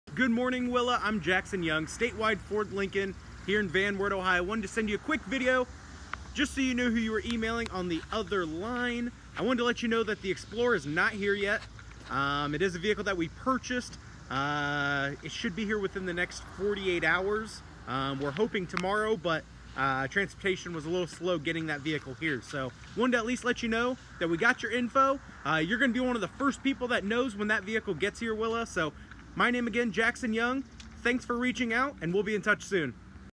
A Video for Willa
Jackson introducing himself to Willa with interest in a 2017 Ford Explorer Sport at Statewide Ford Lincoln